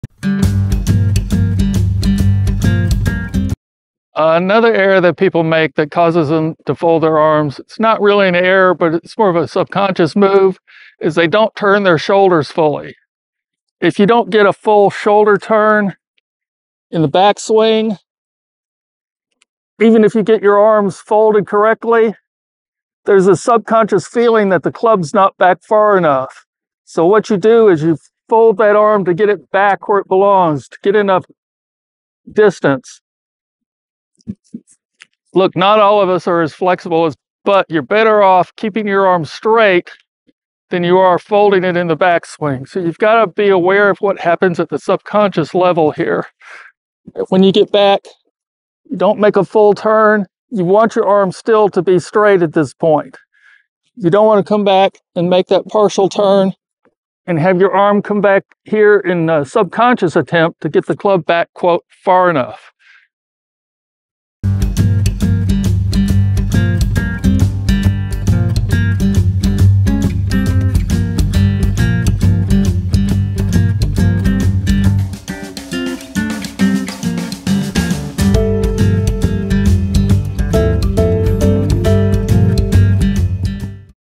Full Video: Avoid Arm Bending for a Better Swing! ep73
Are you struggling with consistency in your golf swing? One of the most common issues that can throw off your game is arm bending during the backswing. But fear not! In this informative video, we'll uncover the errors that often lead to arm bending and how you can correct them to improve your swing.

Join us as we dissect the nuances of the golf backswing and identify the key mistakes that cause golfers to inadvertently bend their arms. From improper posture to overactive wrists, we'll explore each error in detail, providing valuable insights and practical solutions to help you maintain a straight and powerful backswing.

Our experienced golf instructor will guide you through a series of drills and exercises designed to retrain your muscle memory and promote a more fluid, controlled swing motion. Whether you're a beginner looking to refine your technique or a seasoned player aiming to iron out bad habits, this video is packed with actionable tips to elevate your game.

Don't let arm bending hold you back on the course any longer! Let's tee off towards improvement together!